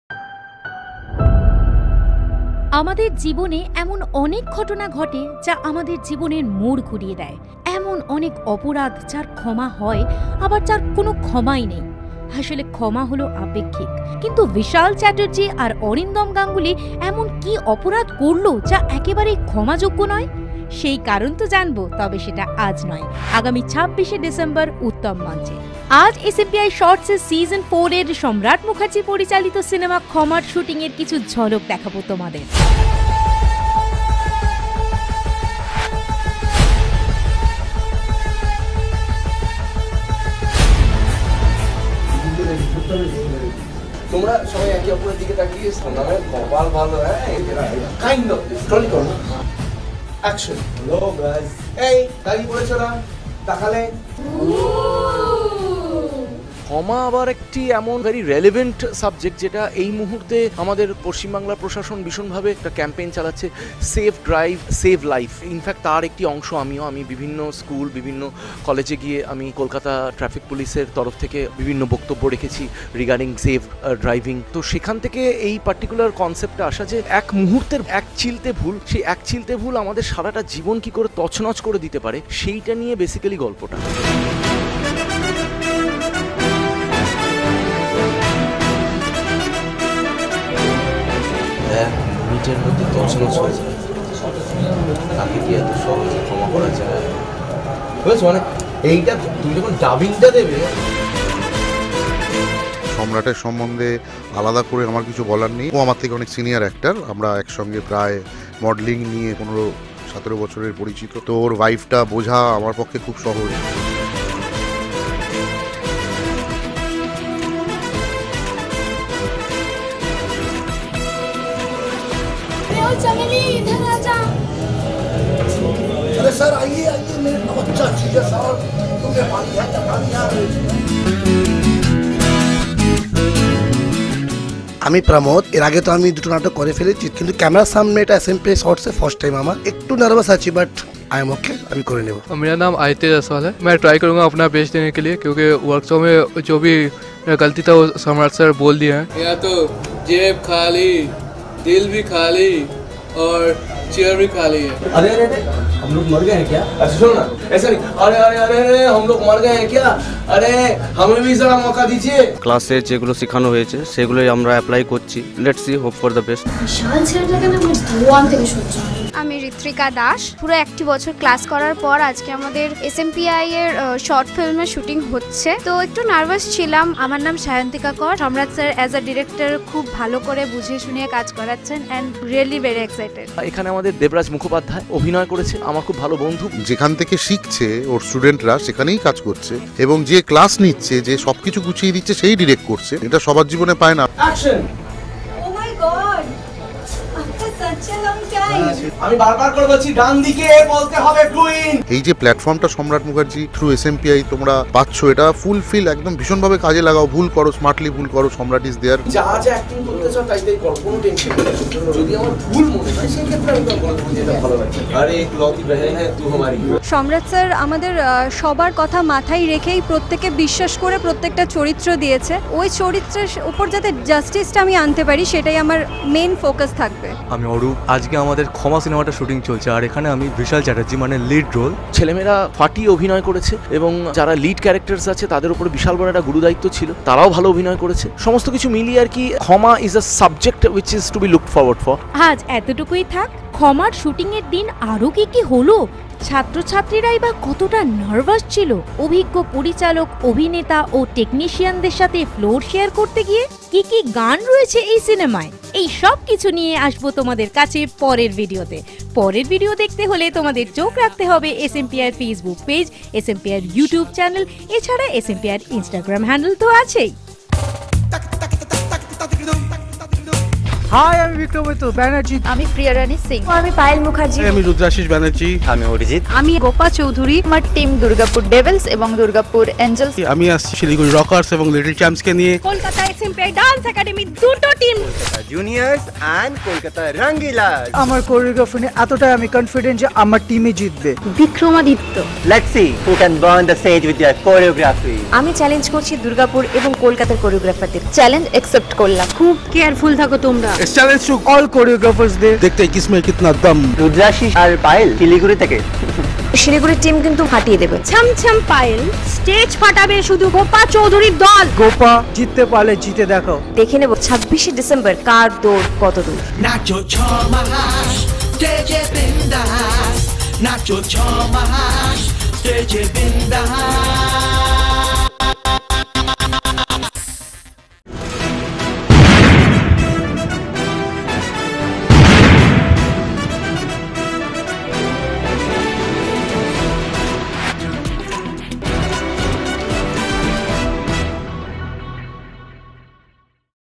SMPAi SHORTS || SEASON 4 || KHOMA || MAKING || SAMRAT MUKHERJI || PART 1 || 2019
Mr.Vishal Chatterjee & Mr.Arindam Ganguly has committed an unpardonable crime!!!

But what was their fault?

Their faults will be unfolded on 26th December'2019 at Uttam Manch.

But before that lets keep our eyes on the making of KHOMA, a diploma film presented by SMPAi SHORTs and Directed by Samrat Mukherji..

ক্ষমার  অযোগ্য অপরাধ করলেন শ্রী ভিশাল চ্যাটার্জী এবং  শ্রী অরিন্দম গাঙ্গুলী!!!
কিন্তু কী এমন সেই অপরাধ?
সেই অপরাধের পর্দা ফাঁস হবে আগামী ২৬ শে ডিসেম্বর’১৯, কলকাতার উত্তম মঞ্চে
তার আগে চোখ রাখা যাক, SMPAi SHORTs প্রযোজিত এবং সম্রাট মুখার্জী পরিচালিত  ডিপ্লোমা ফিল্ম “ক্ষমা”-র সিনেমার শুটিং ফ্লোরে…